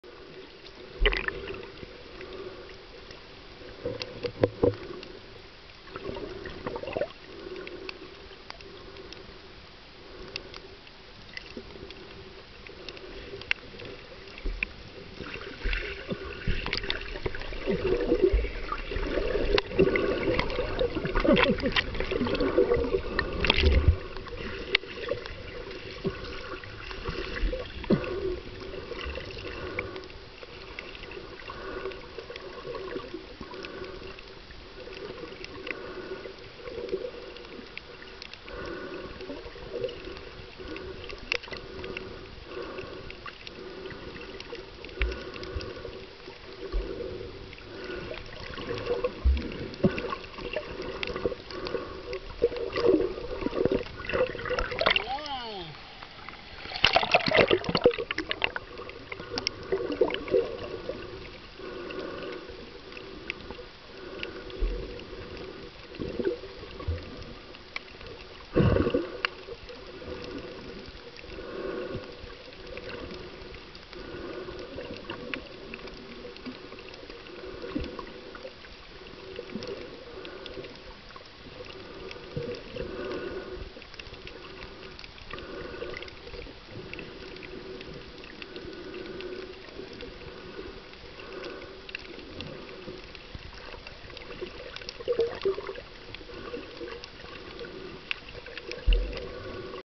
Sea EEL - Sharks Cove Hawaii
Ran into this EEL at Sharks Cove in Honolulu Hawaii